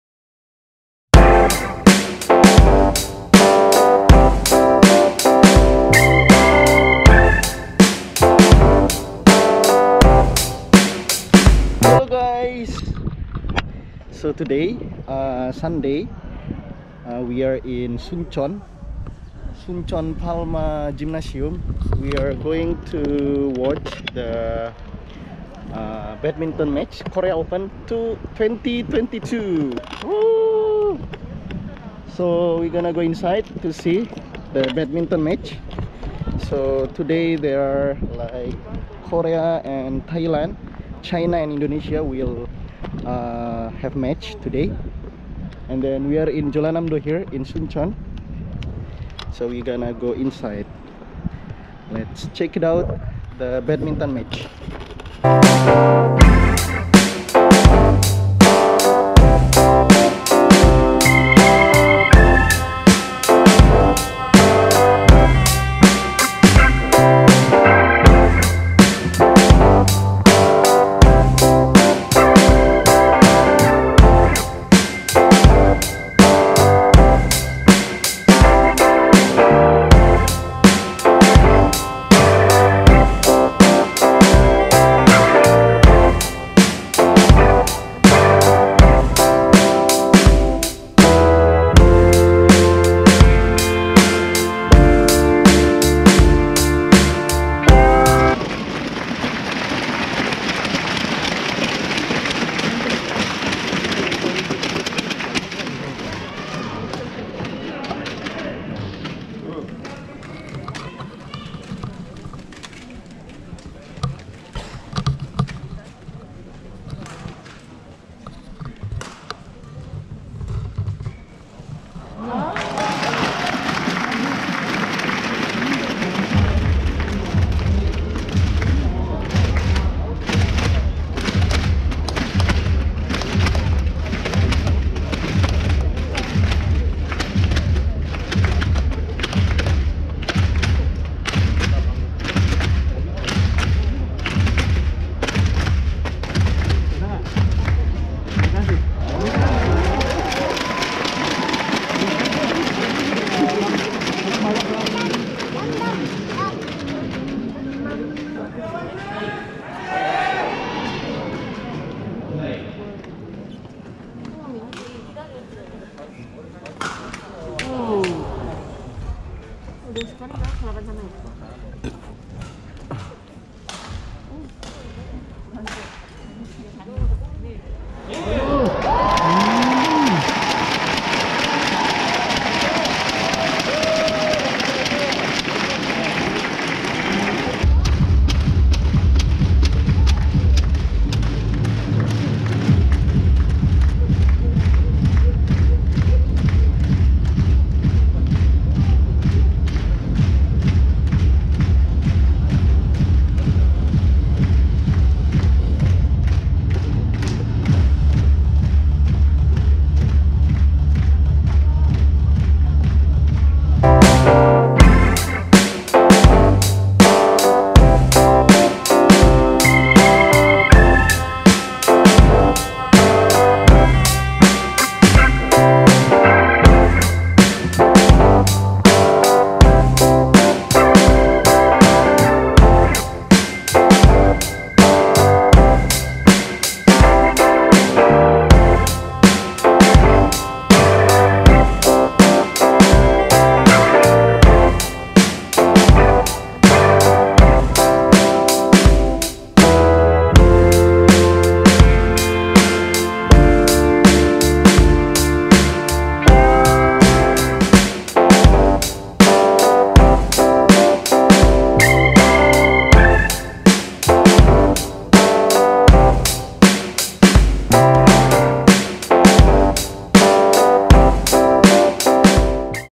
Watching Korea Open 2022 at Suncheon
In order to improve the performance of badminton in Korea and enhance the national prestige through international competitions, the Korea Open Competition has been held every year since 1991 with the world's top players participating.

The Korea Open is an annual badminton event that is commonly held in Seoul, South Korea. The tournament used to be known as Korea Open Super Series because it became one of the BWF Super Series tournaments beginning in 2007. BWF categorized Korea Open as one of the seven BWF World Tour Super 500 events in the BWF events structure since 2018. This year, Korea Open 2022 is at Suncheon, Jeollanamdo!